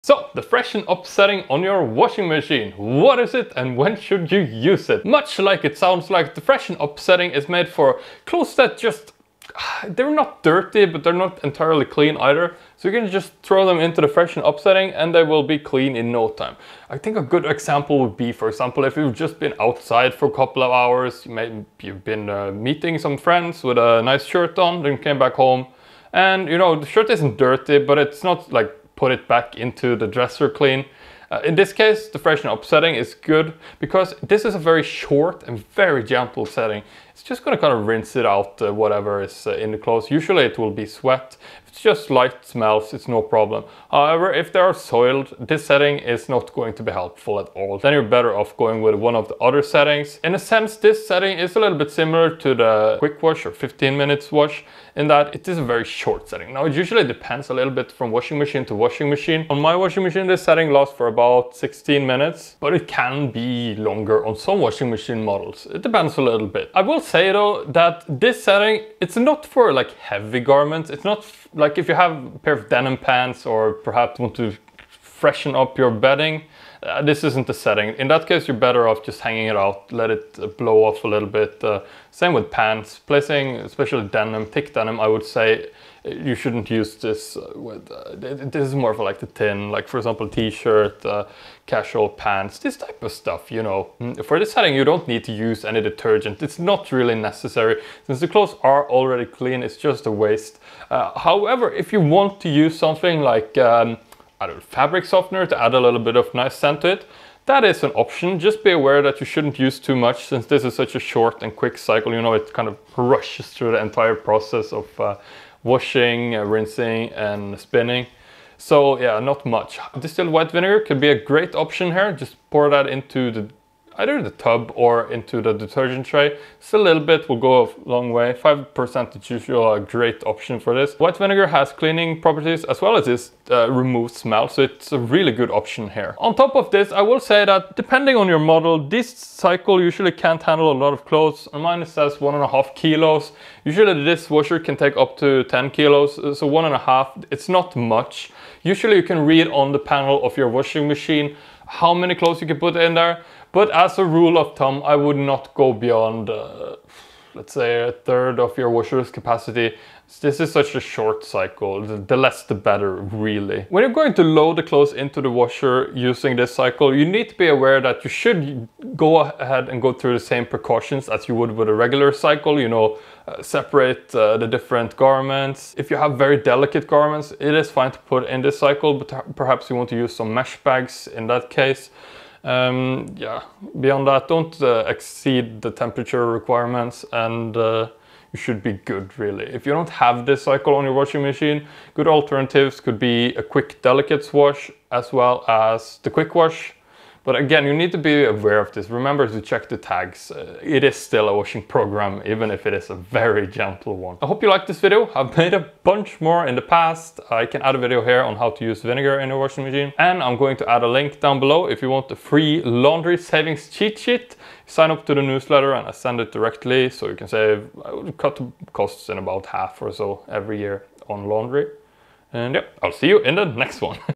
Freshen-Up: An Underrated Washing Machine Program
The Freshen-Up (Or refresh as it's called on my washer) is a fantastic program to have available if you have any clothes that are just a little used, but you don't deem them clean enough to put them back in the closet with your completely clean clothes.